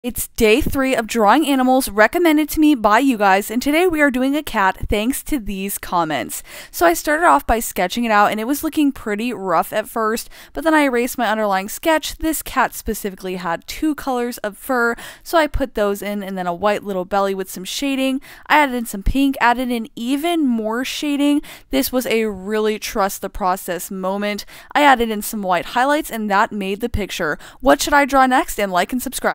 Day 3 Drawing Animals YOU Recommend 🐱
It’s day 3 of drawing animals recommended to me by you! Today we have a cat drawn with Ohuhu markers! What animal should I draw next?